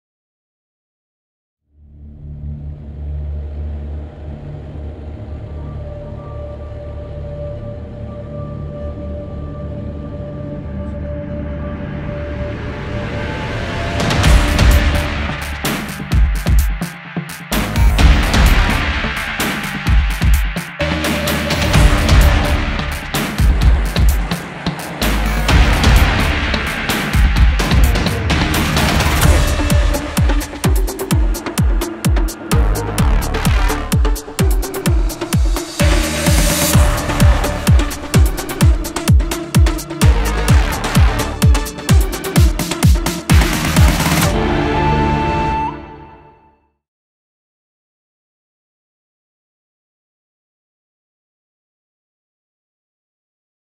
Volvo G-series articulated hauler: The perfect package
Rely on continuous production with the strong and durable G-Series articulated haulers from Volvo. Featuring a heavy-duty hitch, front and rear frames and wet disc brakes, the G-Series combines strength and durability to increase uptime. And with the support of an extensive range of maintenance options, you'll never have to worry about getting the job done.